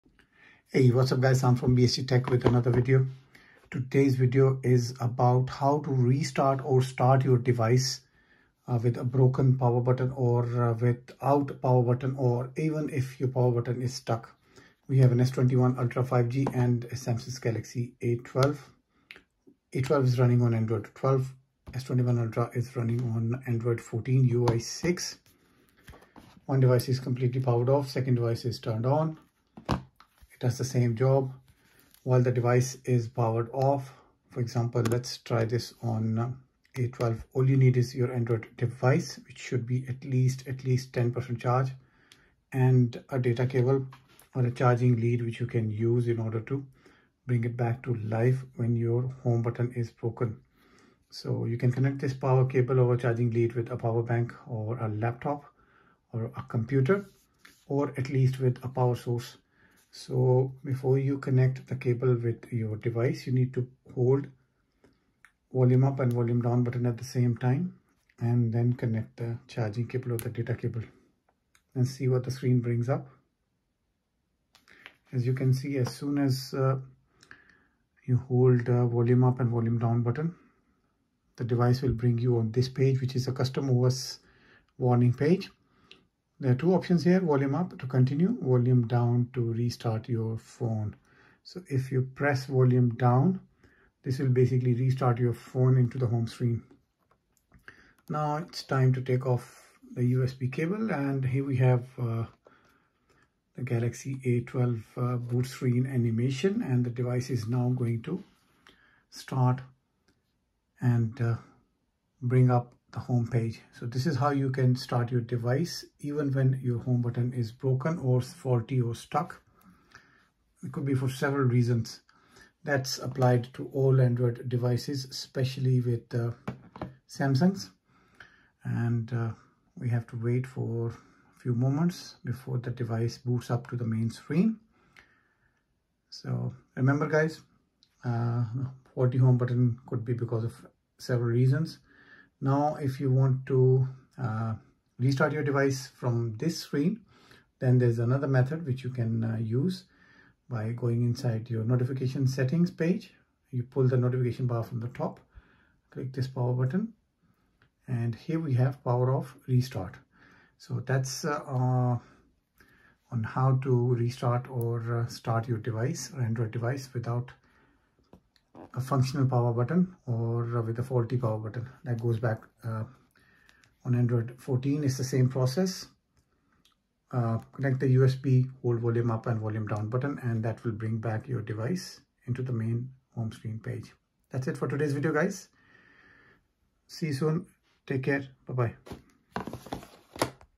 how to power on or restart android SMARTPHONE with broken power button
how to power on or restart android with broken power button